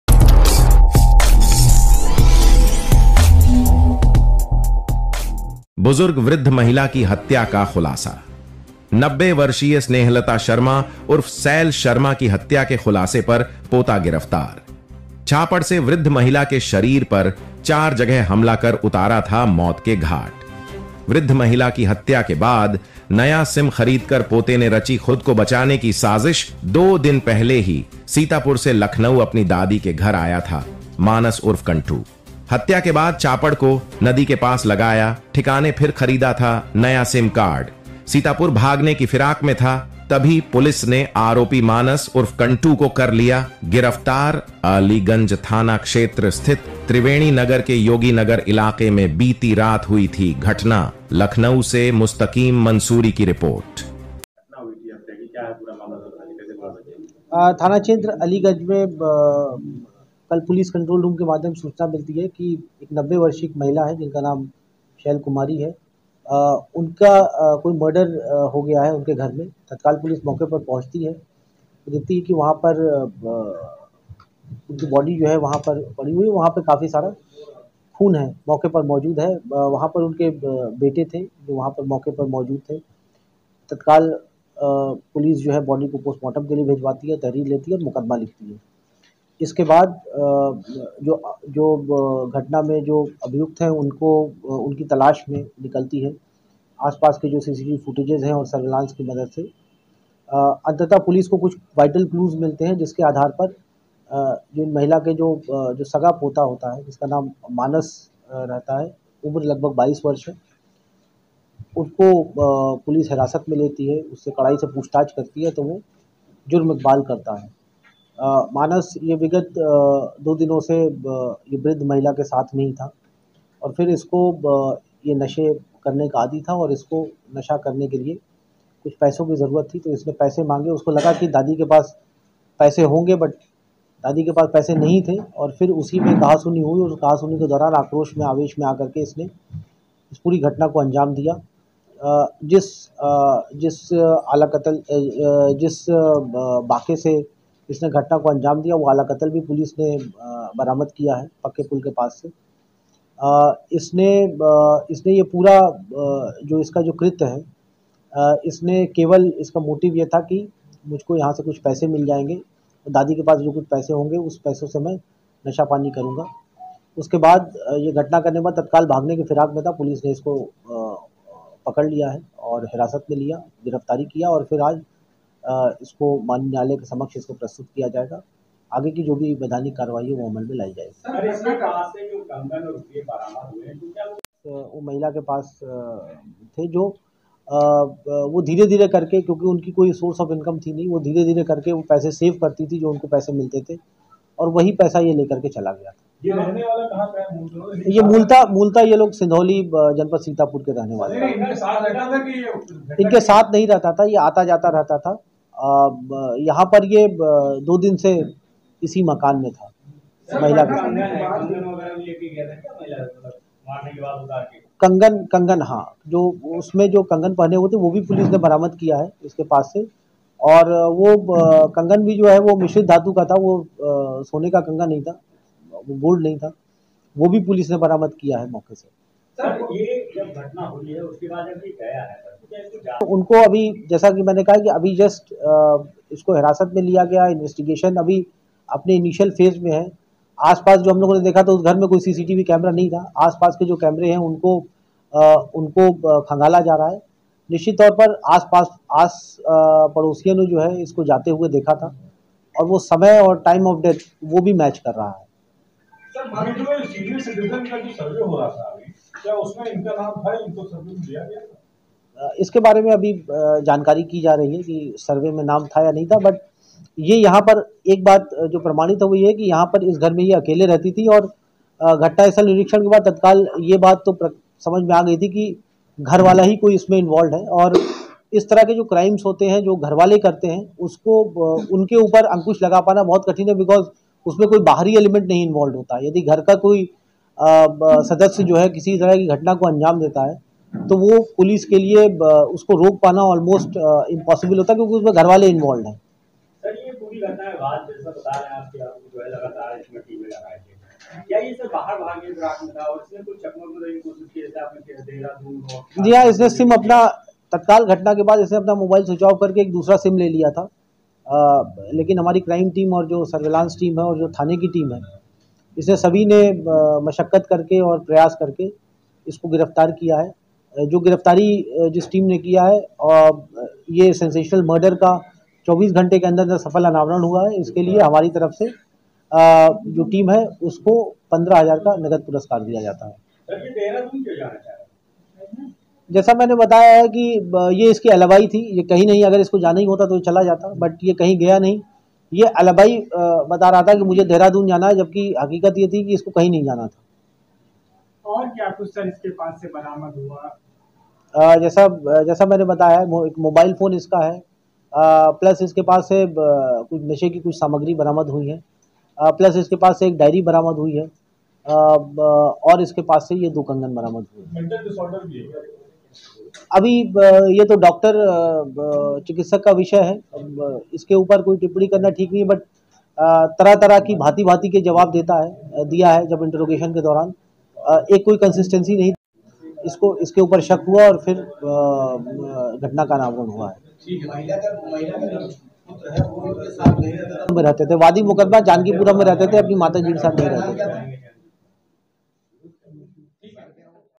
लखनऊ, बुजुर्ग वृद्ध महिला की हत्या का खुलासा हत्या के खुलासे पर पोता गिरफ्तार
बुजुर्ग वृद्ध महिला की हत्या का खुलासा हत्या के खुलासे पर पोता गिरफ्तार